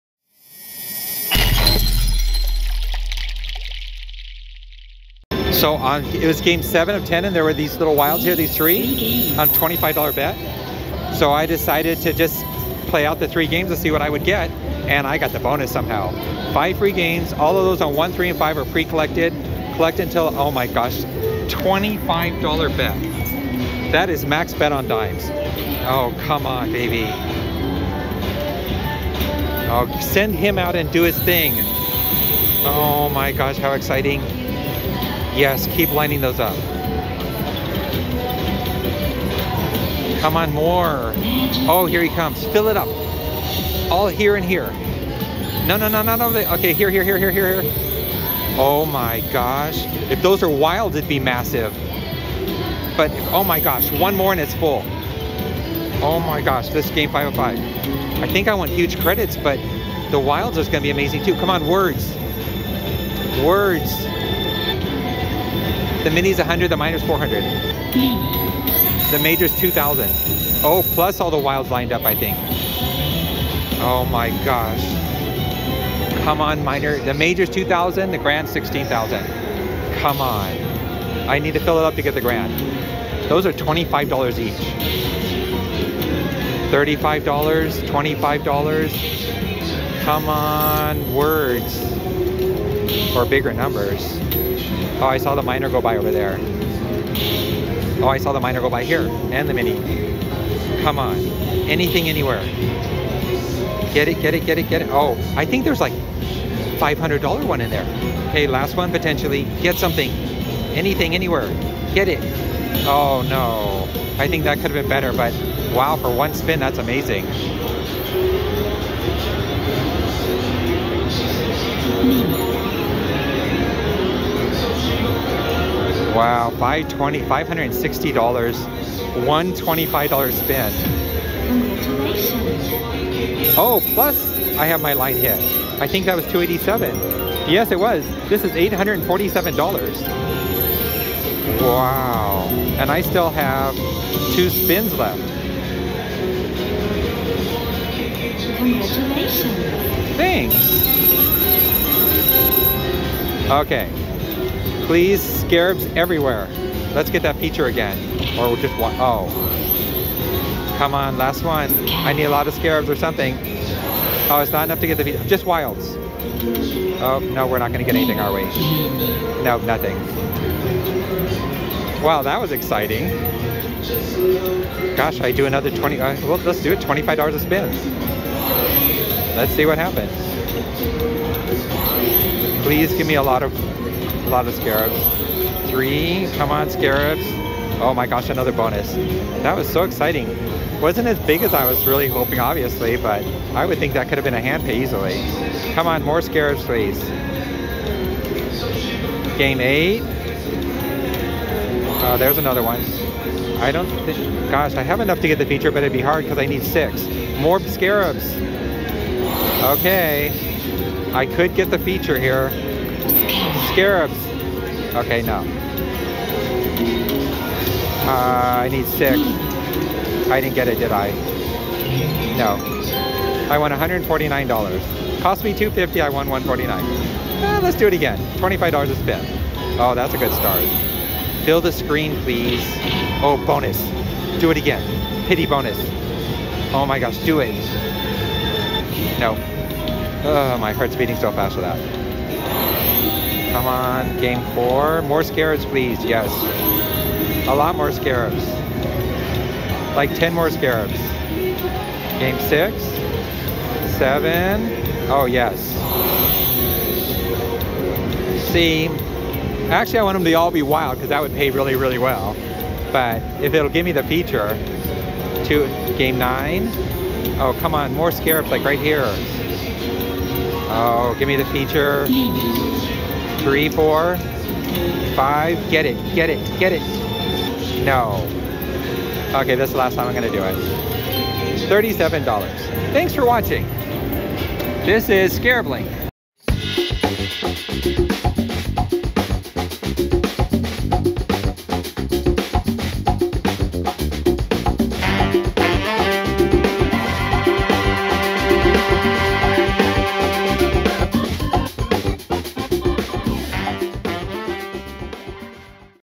CRAZY $25.00 BET FREE GAMES ON SCARAB LINK - GREAT WIN!!!
I found several scarabs left by another person who was playing $25.00 A SPIN and while spinning to see if I could fill up the screen I was totally Shocked and Surprised that I got the Free Games on this Crazy Big Bet!

All Bonus Fun on Random Games Featuring:
Scarab Link Slot Machine Pokie

Sometimes we get Big Bonuses and sometimes we get Small Bonuses and that's what this video is all about!!!

We Truly Hope You Enjoy This Video!!!